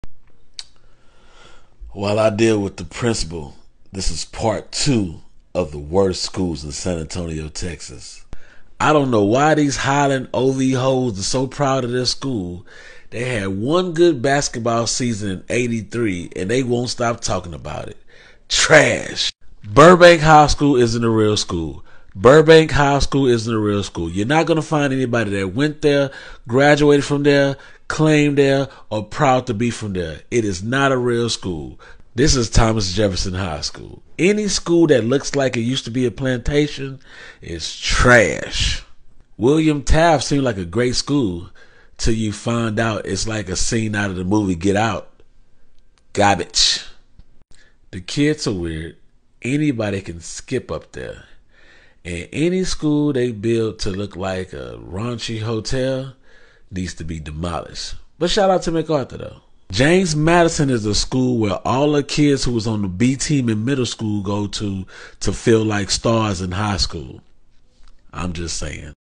Worst High Schools in sanantonio pt2
Comedian Alvin Perry takes you thru the educationalsystem of sanantonio to find out what districts have the worst highschool pt2 😂 This is satire and not to be taken seriously